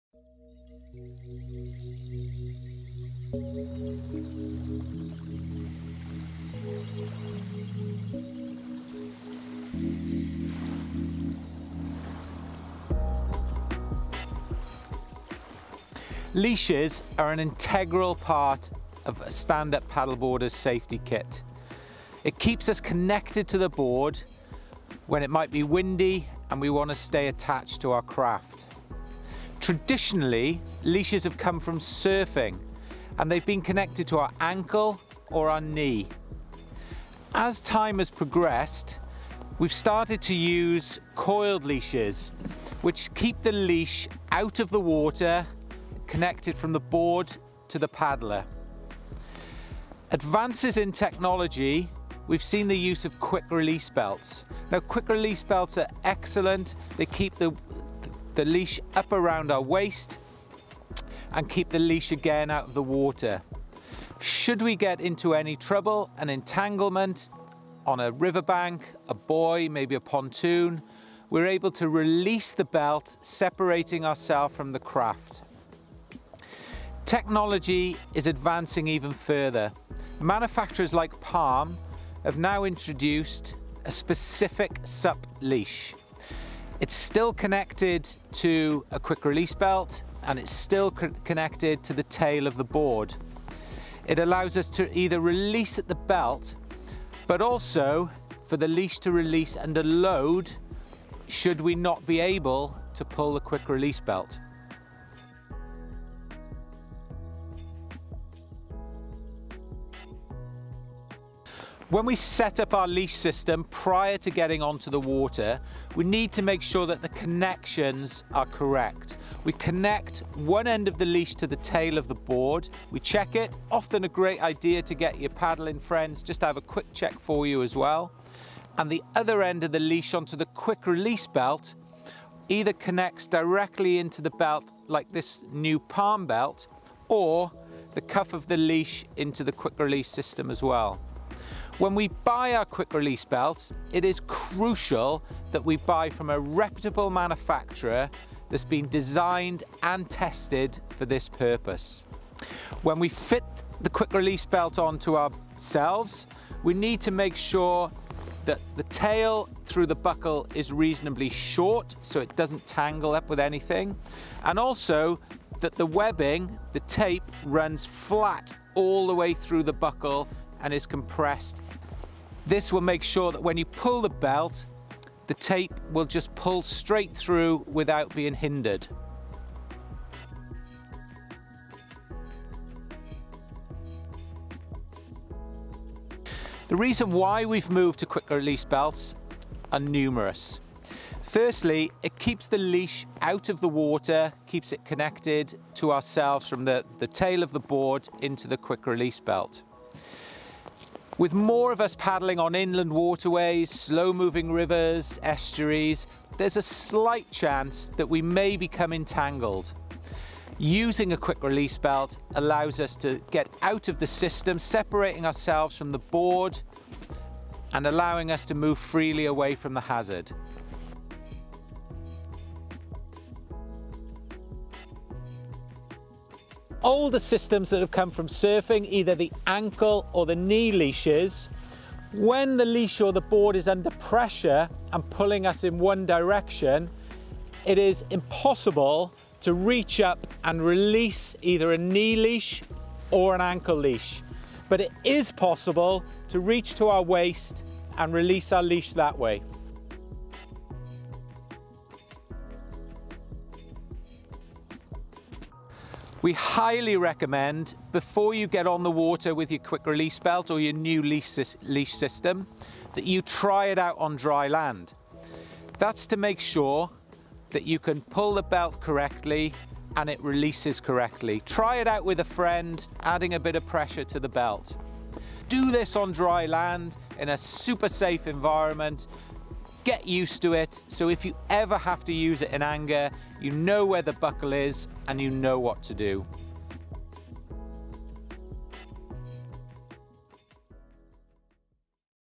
Demonstrating SUP Quick Release Leashes & Waist Belts | Water Skills Academy X Palm Equipment
With more of us paddling on inland waterways, slow moving rivers and estuaries, there's always a chance that we may become entangled. Using a SUP quick release belt allows us to get out of an entanglement situation quickly, separating ourselves from the board and allowing us to move freely away from the hazard.

All the connective systems used in SUP that have come from surfing, such as the ankle or knee leashes, have demonstrated that when the leash or the paddleboard is under pressure and pulling you in one direction, it's near impossible to reach up and release either a knee leash or an ankle leash.

Just wearing a QR belt is not enough, follow manufacture's guidance, get training and practice, practice, practice. Here's our demonstration of how to correctly fit and wear the new Palm Equipment SUP quick release leash and waist belt system, launching this summer.